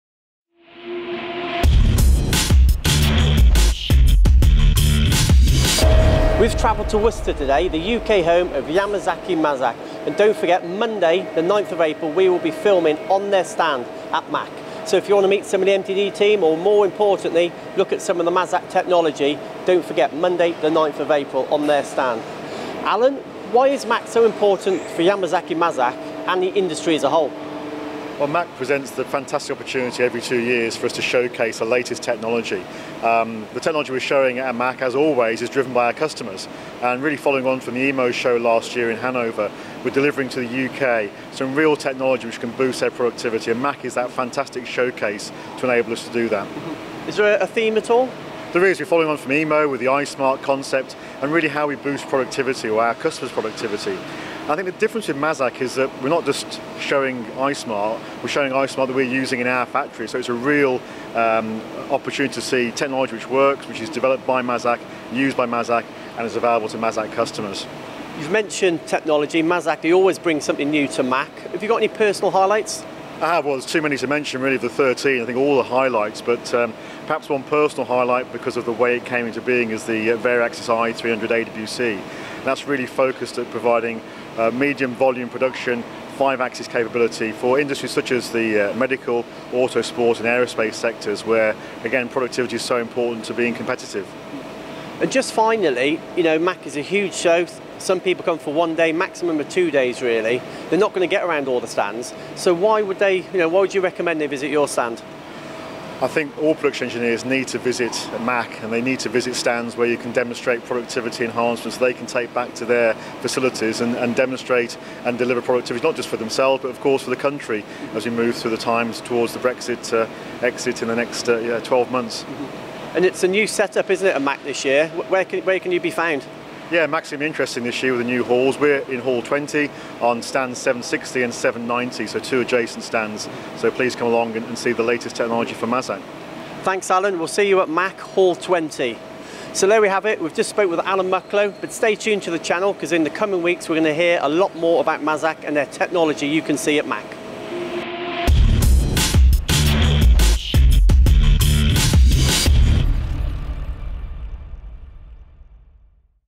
Mazak MACH 2018 / MTDCNC film live on their stand Monday 9th April
Listen to what Mazak will be showing at MACH 2018 as Joe talks to Alan Mucklow in the first of many preview videos from MTD relating to Mazak’s appearance at the event.